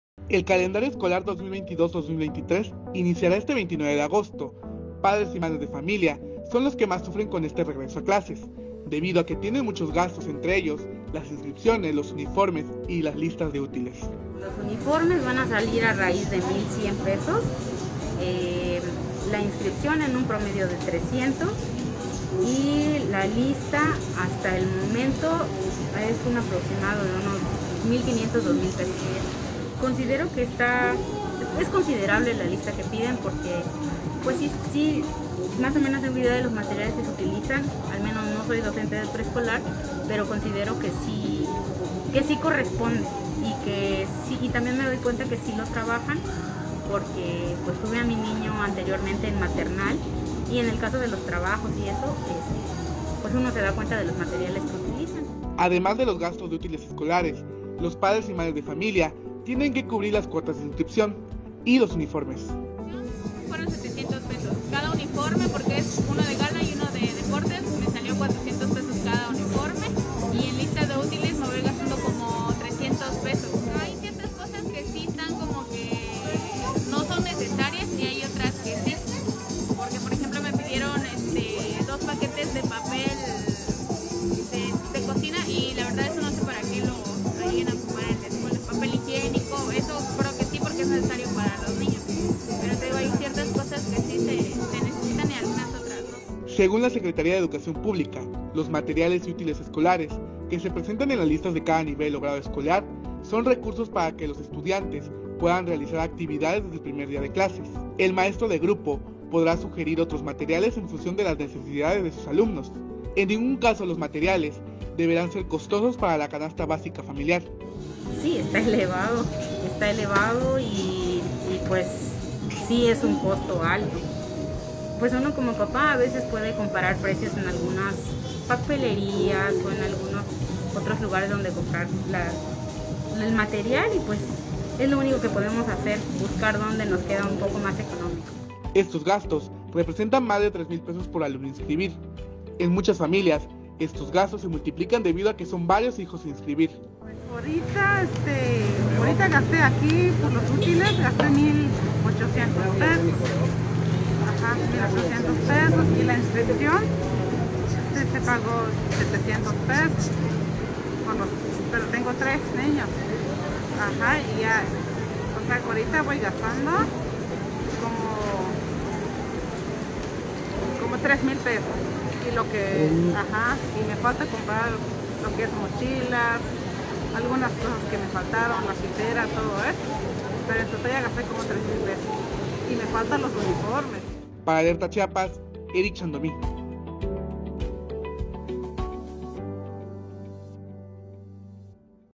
Agosto es la otra cuesta del año; regreso a clases trae varios gastos
Además de los gastos de útiles escolares, los padres y madres de familia tienen que cubrir las cuotas de inscripción y hasta compra de uniformes.